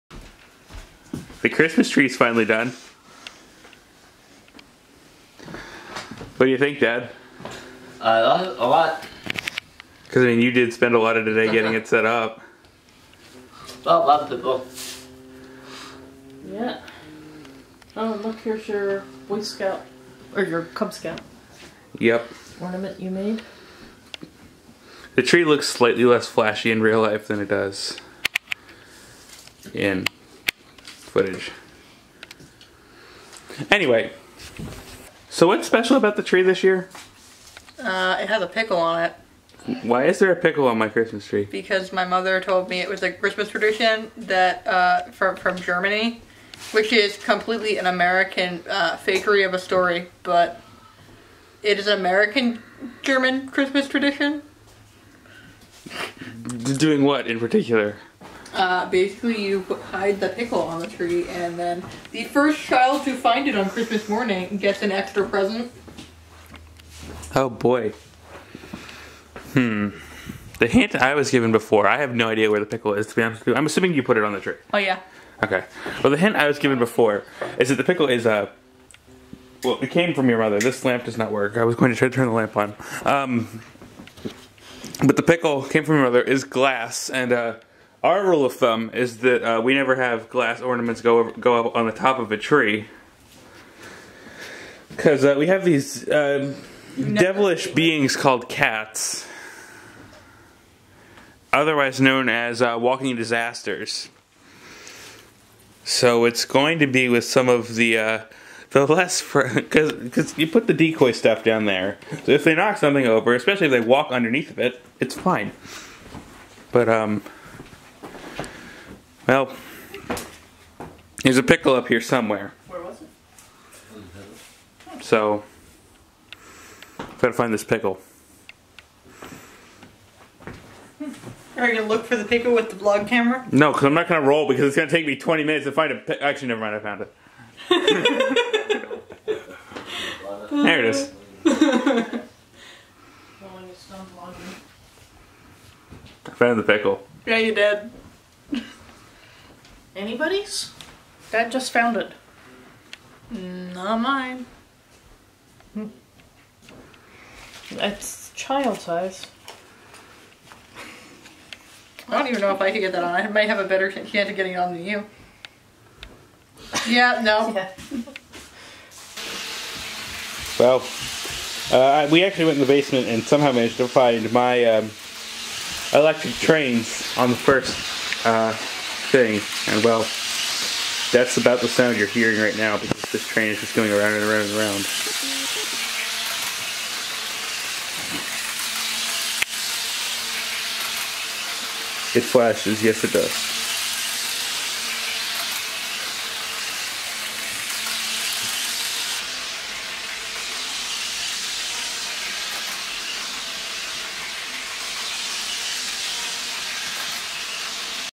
Find This Pickle - Day 1100 | December 24, 2017
Deranged Pennsylvanian man gets confused by a pickle on his Christmas tree.